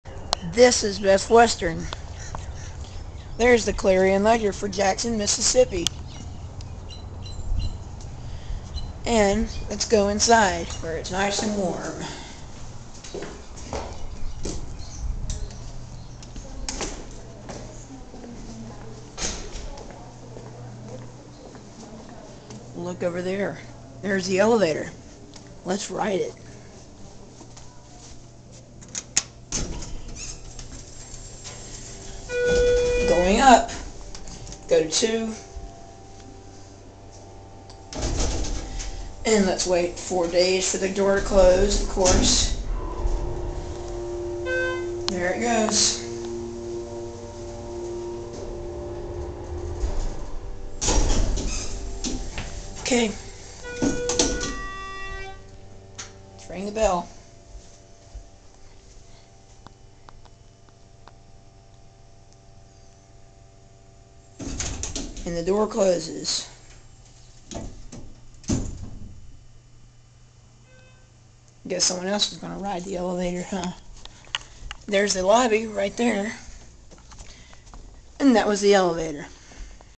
schindler elevator at best western
this is the newer model of a schindler elevator. Plz rate and comment